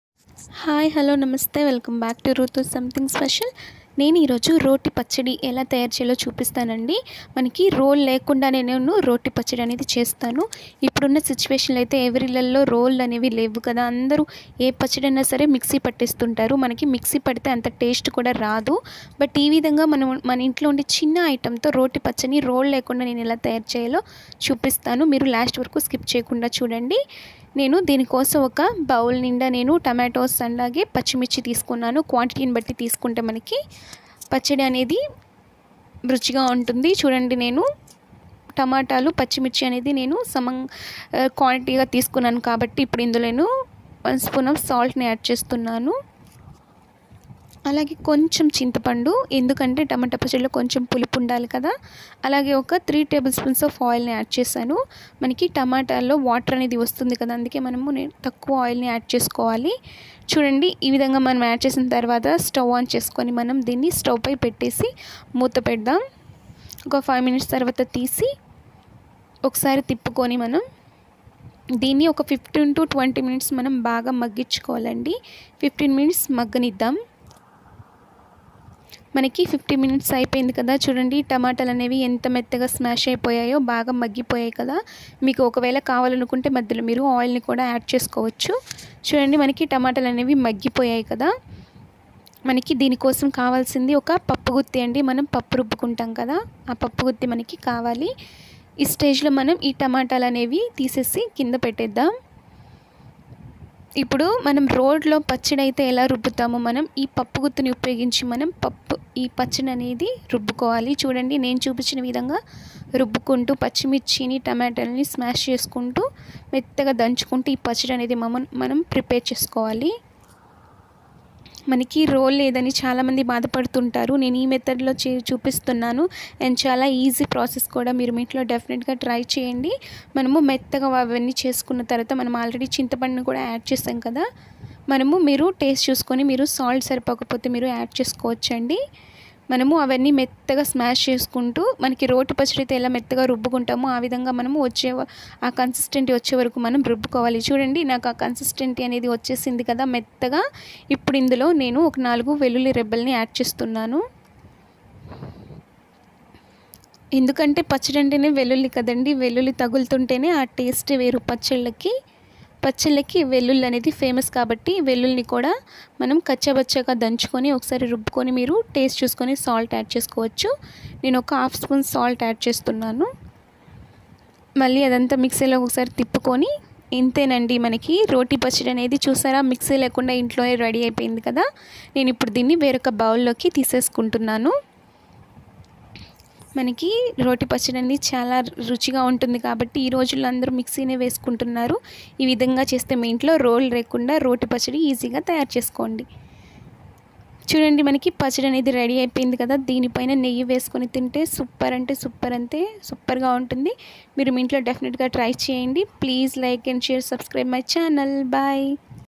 Tomato Roti Pachadi||రోటీ పచ్చడి|| రోలు లేకుండానే చేయొచ్చు ఇలా కనుక చేసారంటే పచ్చడి రెడీ👌👌👌
Roti Pachadi|| roll Le Kunda Ne intilone tayaru  chesukovachu || pachaddi red 👌